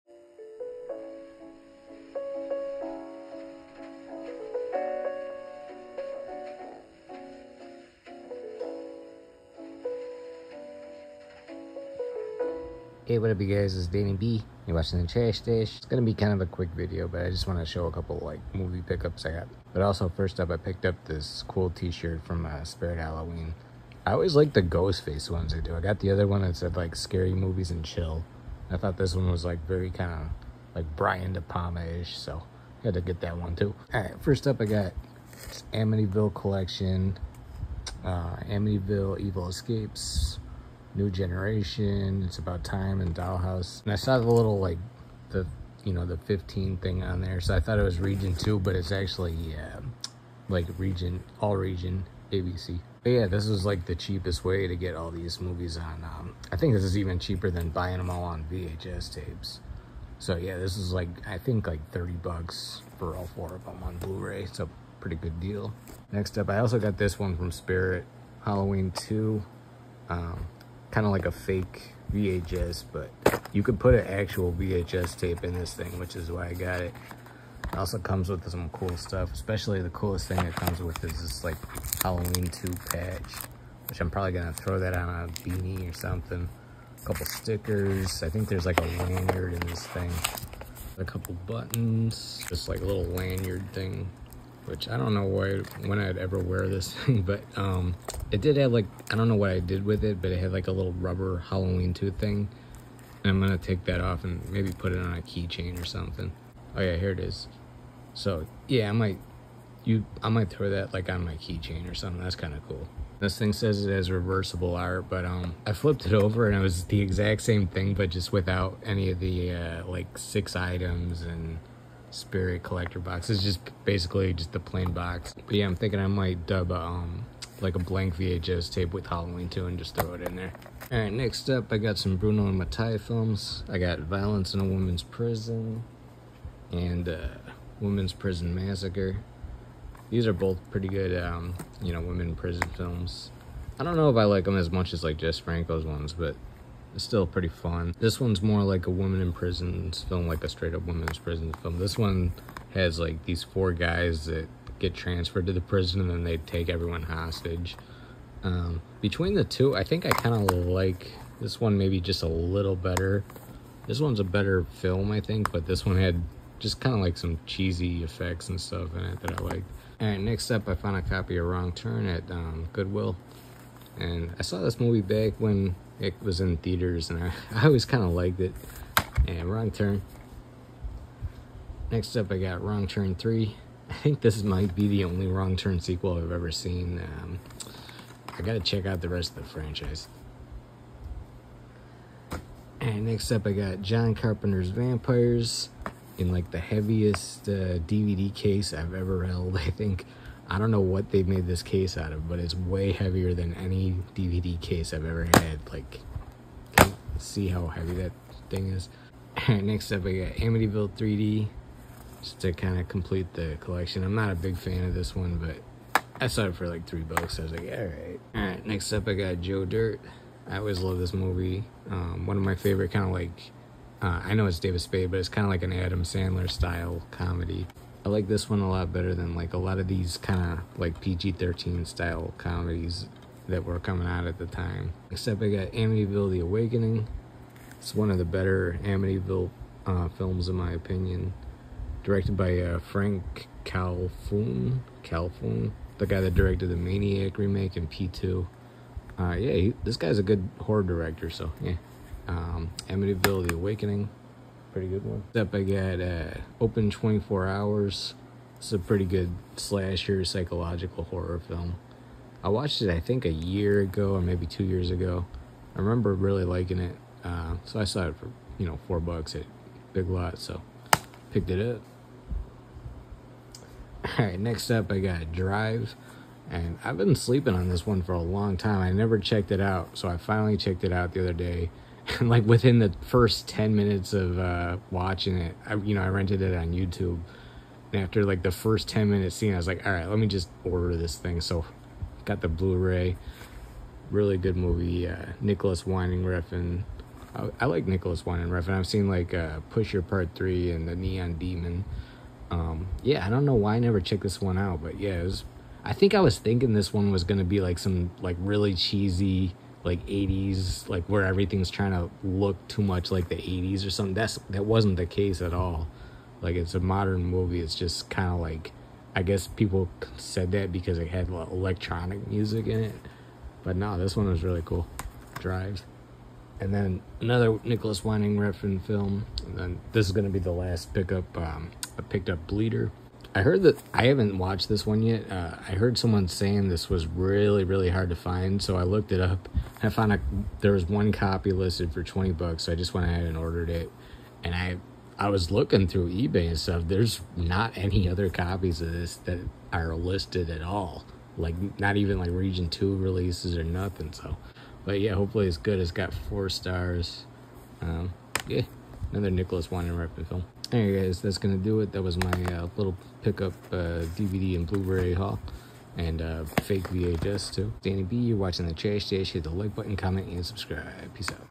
Movie Pickups! Spirit Halloween Edition! 💥Blu Rays! Dvds! Vhs?💥
some new movie pickups i got, plus a t shirt and a vhs collectible from spirit